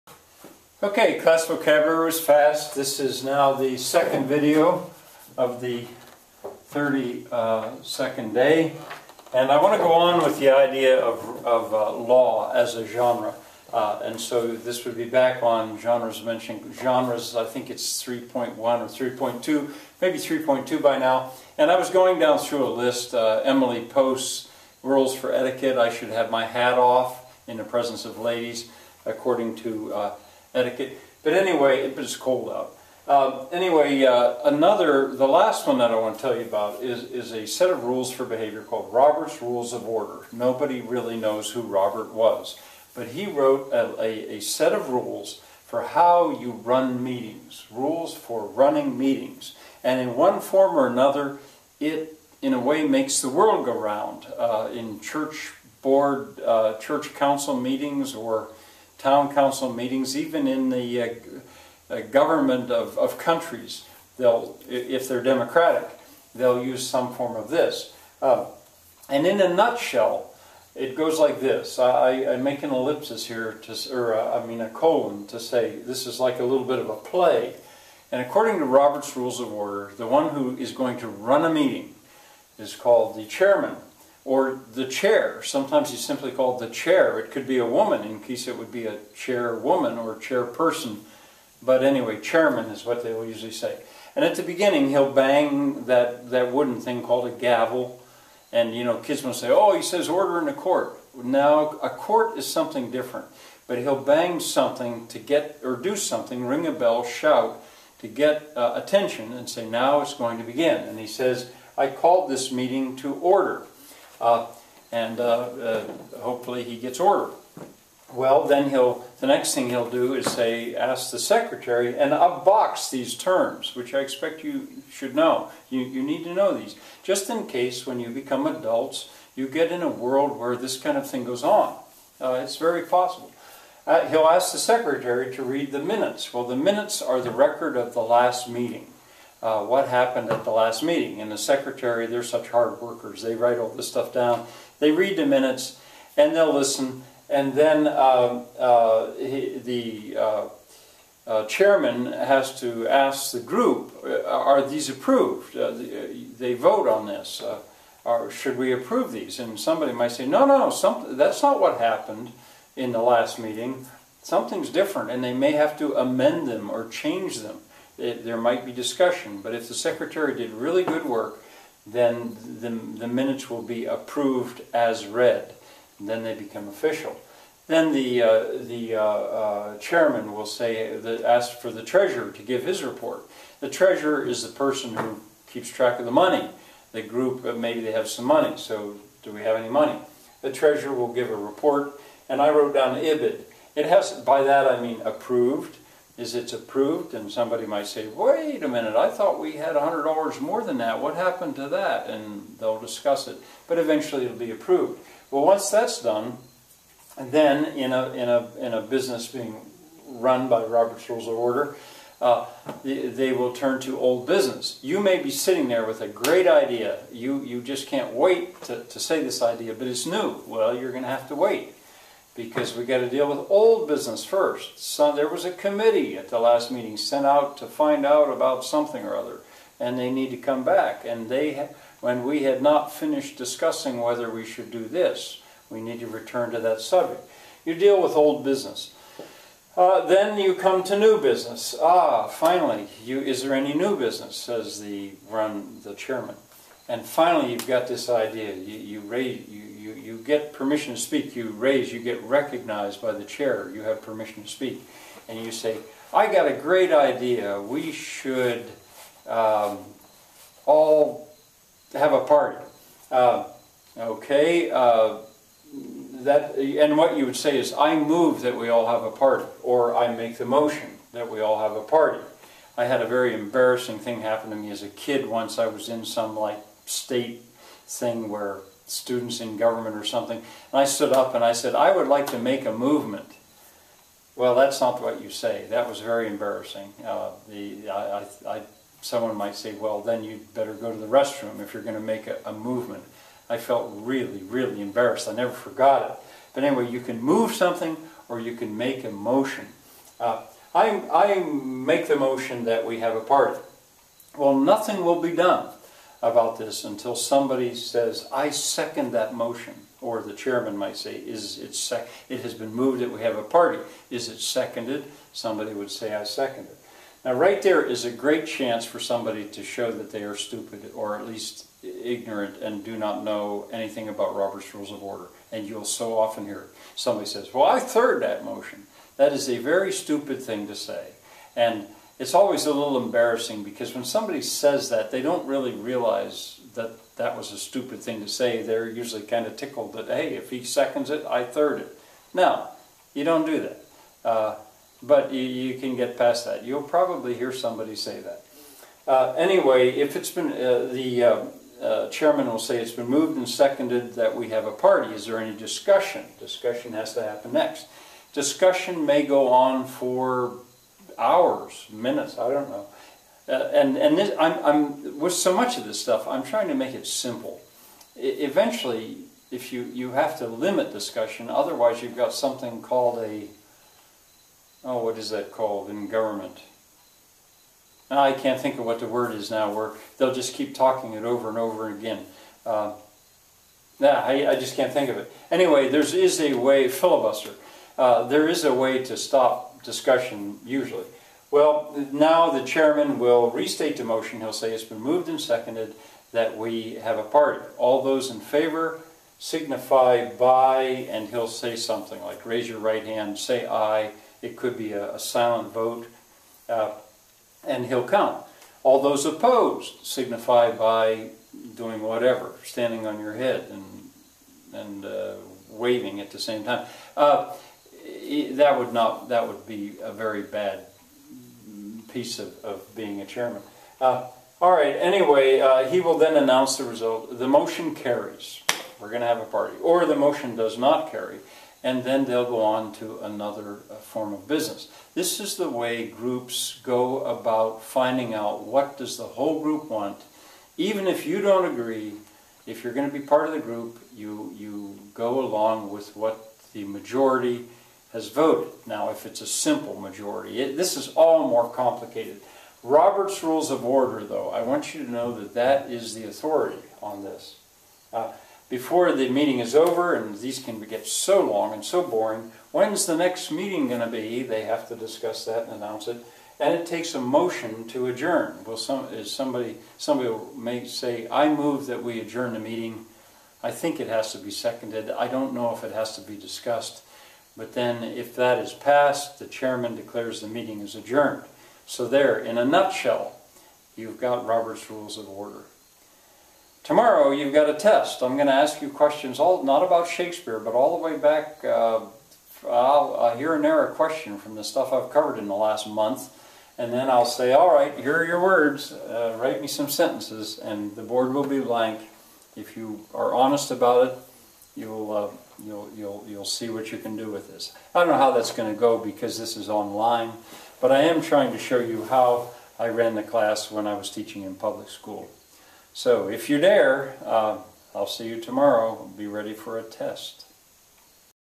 1.32.2 Robert's Rules of Order
Frank gives a quick version of how meetings are run according to parliamentary procedure.